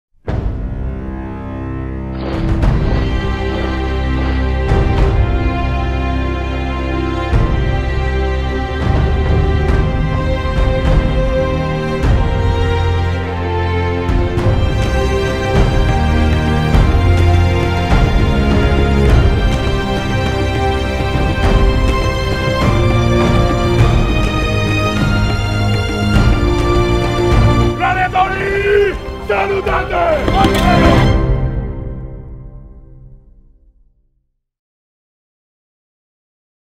Ubrique vuelve a ser Romano.
Este Fin de Semana del 2, 3 y 4 Junio, VIII Bajada Romana de Ubrique 2017.